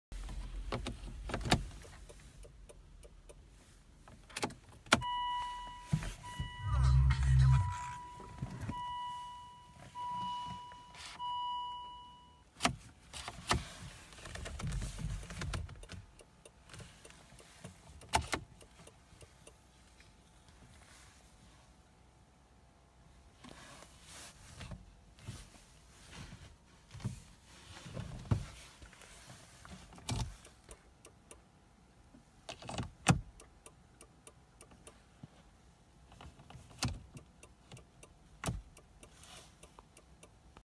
Volvo v50 steering lock issue, SOLVED
Solved.
It was steering column lock issue. Sometimes this error shown because of ignition lock fault.
Repaired old one column lock.